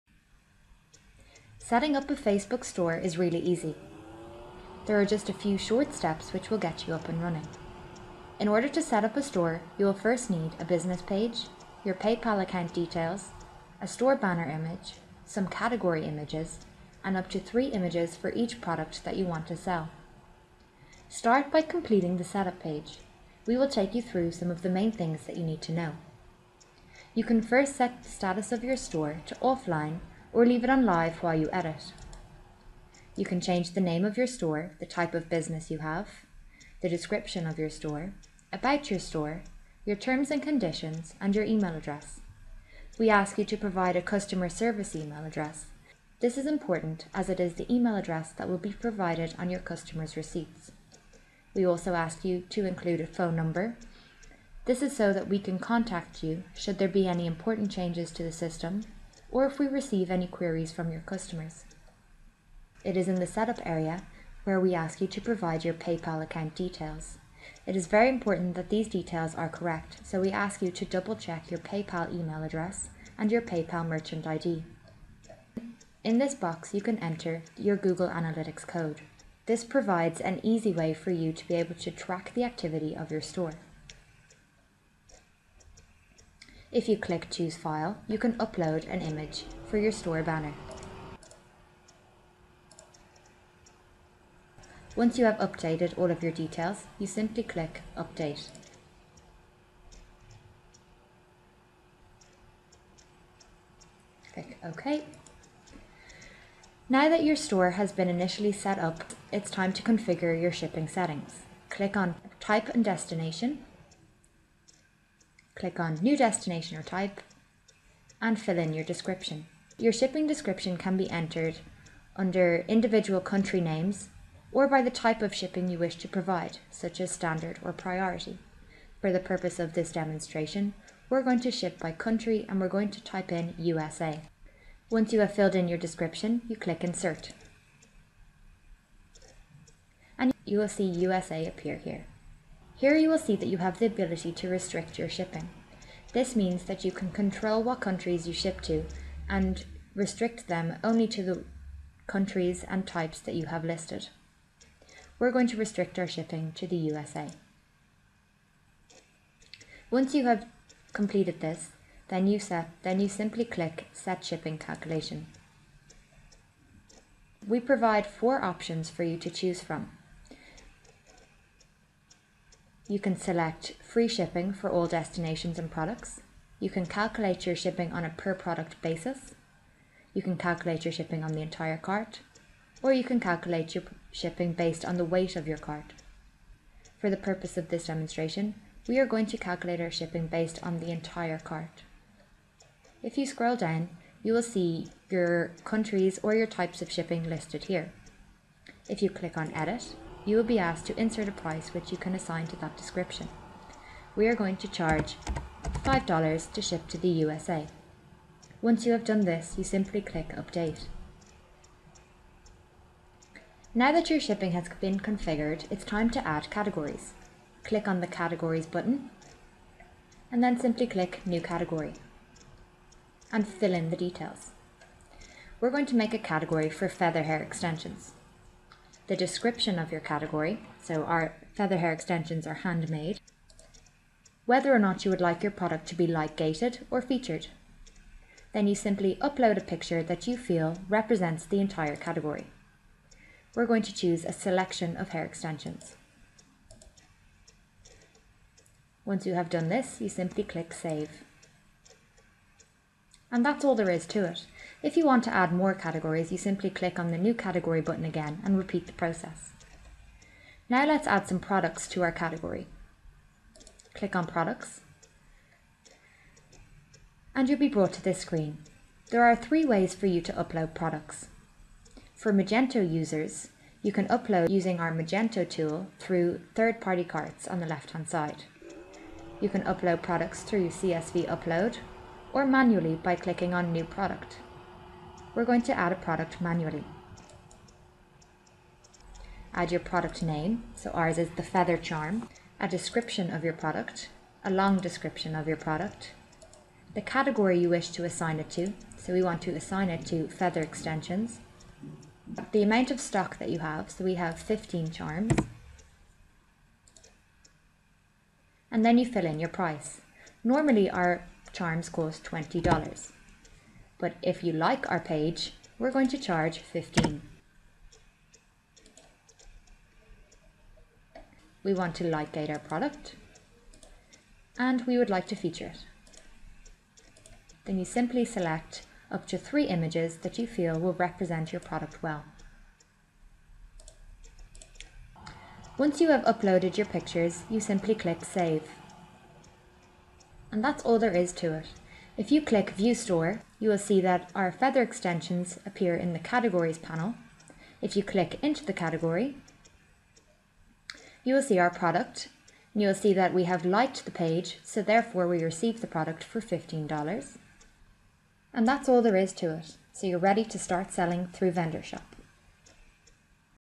Tutorial: How to set up a VendorShop Store
Tutorial video from Vendorshop social on how to set up our f-commerce shopping cart app. You can have your free Facebbok shop in minutes.